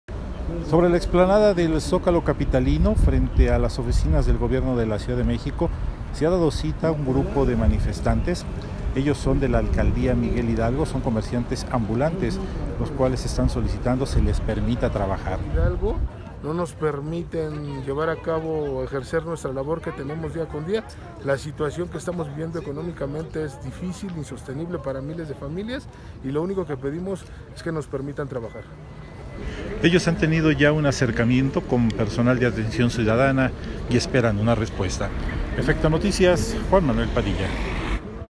Comerciantes de MH piden se les permita trabajar
Comerciantes ambulantes de la @AlcaldiaMHmx han llegado frente a las oficinas de @GobCDMX para pedir se les permita trabajar.